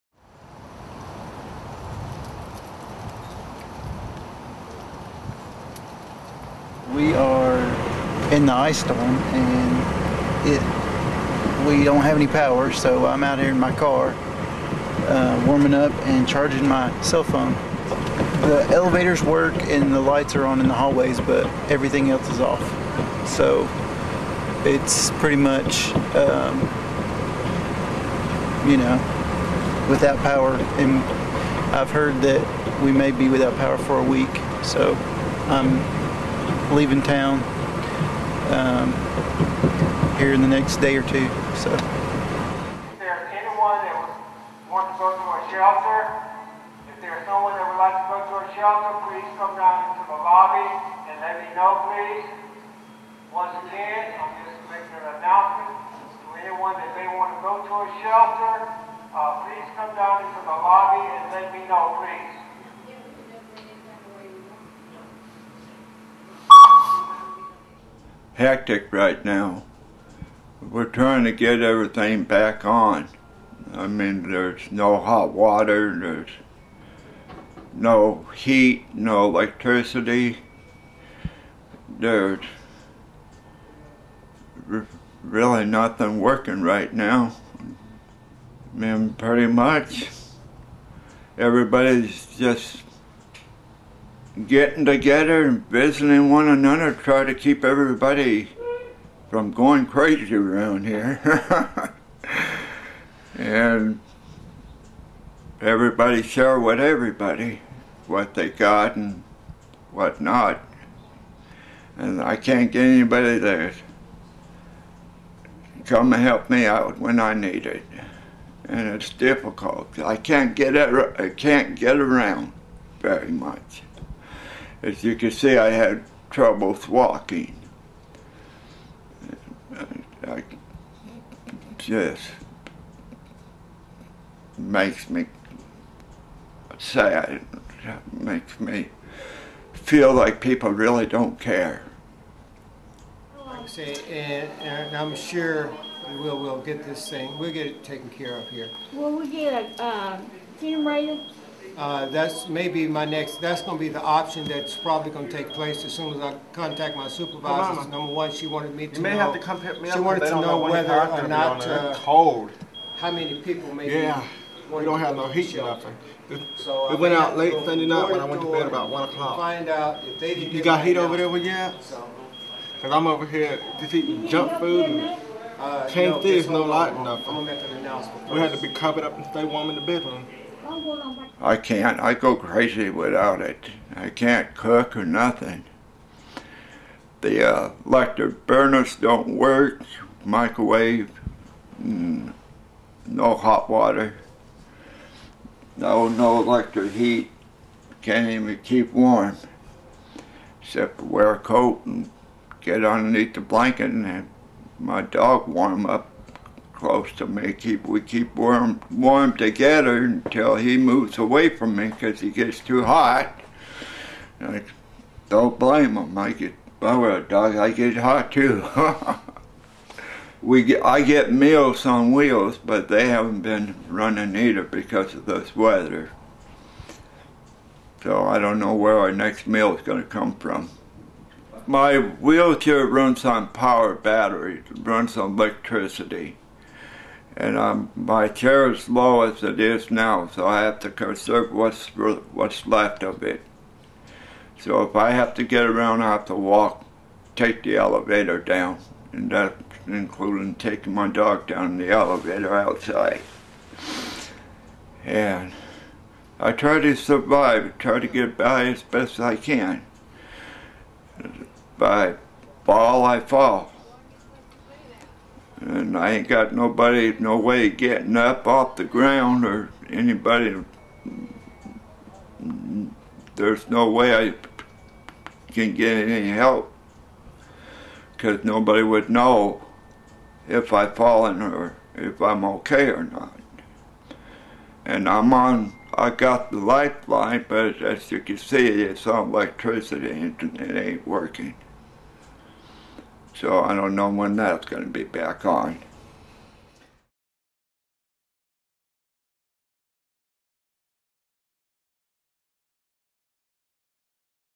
Ice storm at Murdock Villa - tulsaworld.com
Murdock villa, a center for people with physical disabilities, lost power during an Oklahoma ice storm.